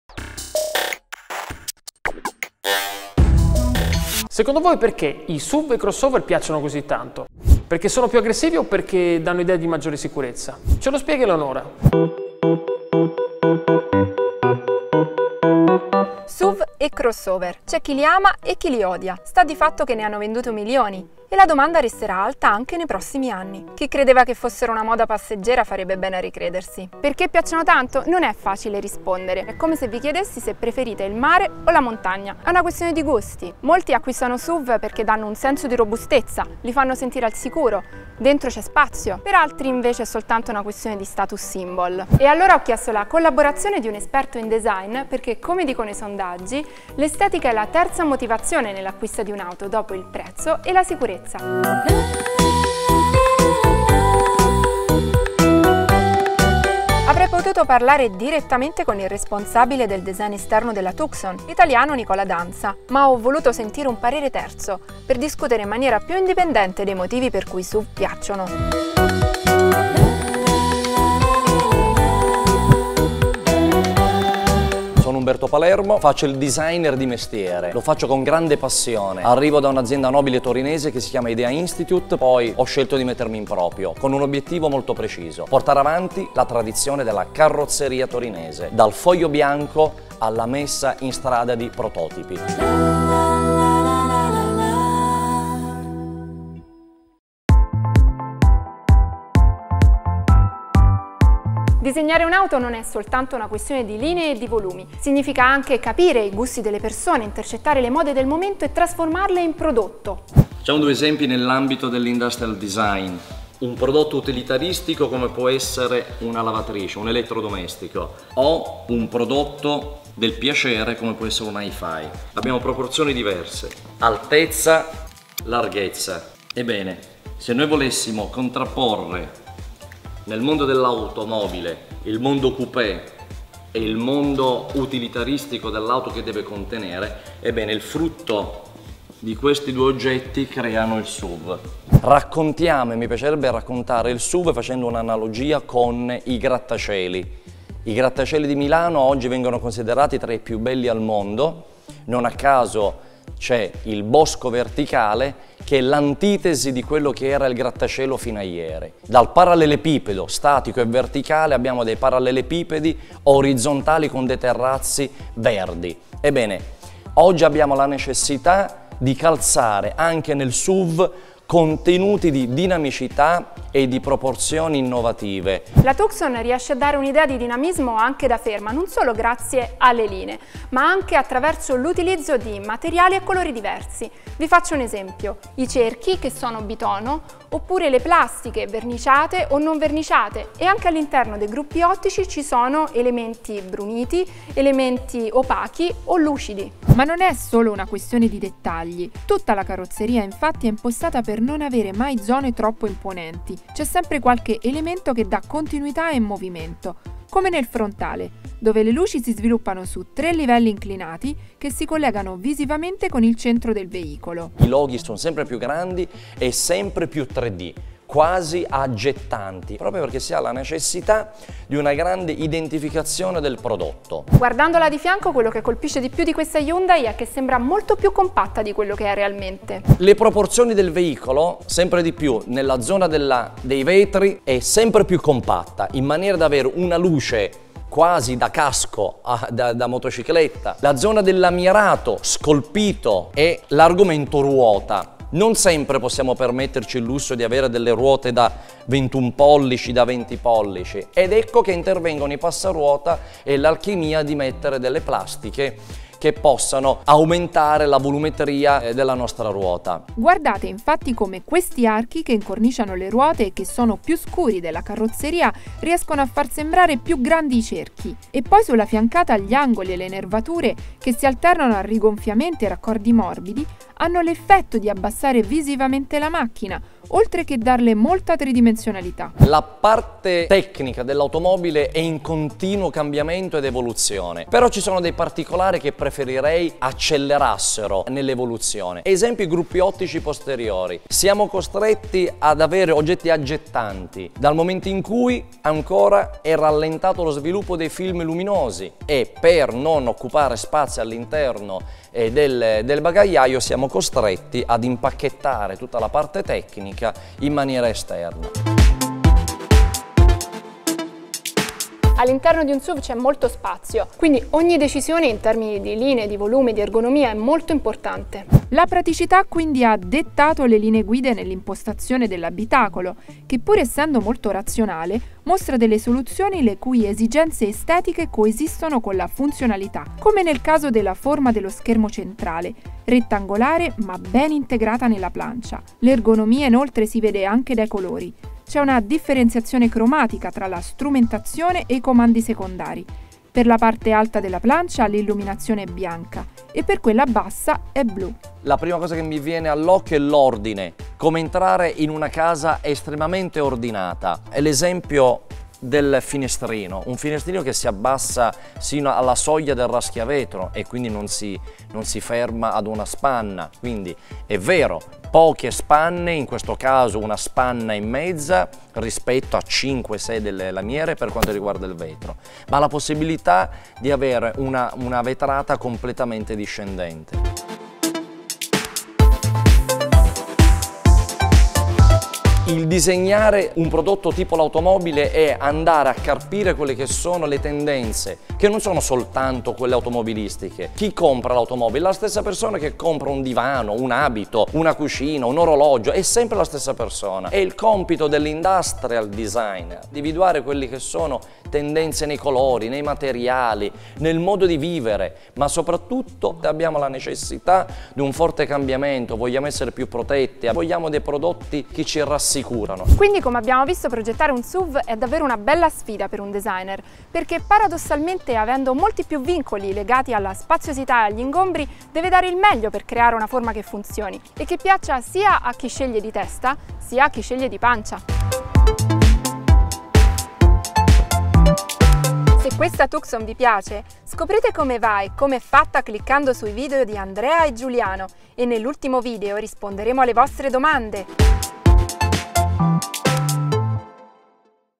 Hyundai Tucson I Quanto conta il design di un'auto
Il segmento dei SUV è quello che negli ultimi anni non ha conosciuto crisi, anzi è cresciuto sempre di più e continua ad avere successo. Come mai? La risposta l'abbiamo cercata indagando sul design, perché quando si compra un'auto la bellezza è un driver motivazionale fondamentale, insieme al prezzo e alla sicurezza. Per saperne di più

CREDITS
Servizio di: OmniAuto.it
Riprese: Edimotive
Editing: Edimotive
Produzione: Edimotive